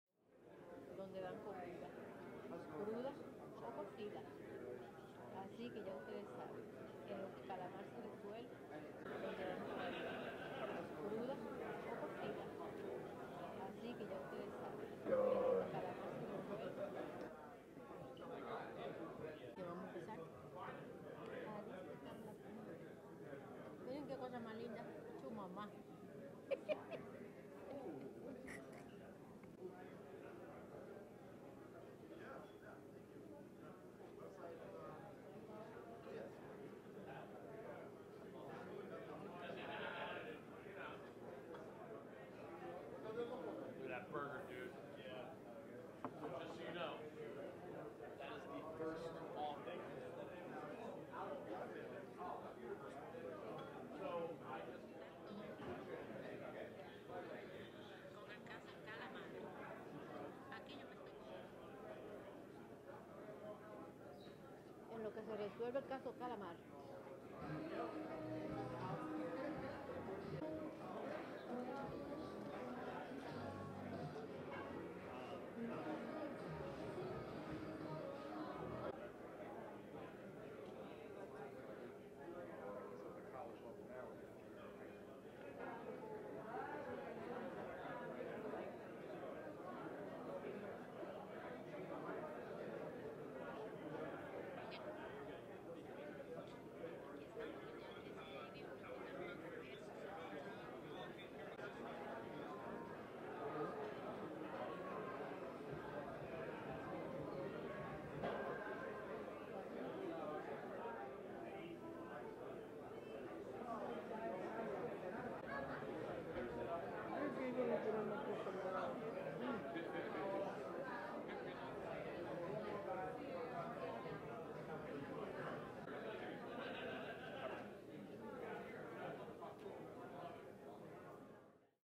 EL nuevo MENU de comida de LOS YANKEES en 2023 para los fanáticos
NY YANKEES PRESENT THEIR 2023 NEW FOOD MENU, WHICH WILL BE AVAILABLE TO ALL FANS ATTENDING GAMES